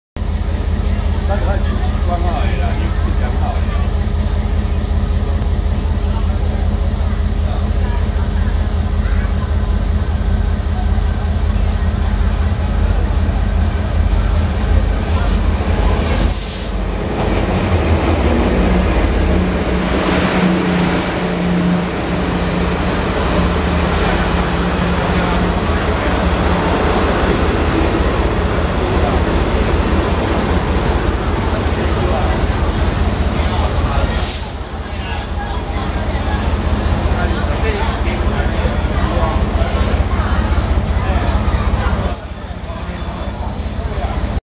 台鐵704次莒光號進站(知本)
台鐵704次莒光號進站(知本),攝於2015年8月23日,3671次普通車交會~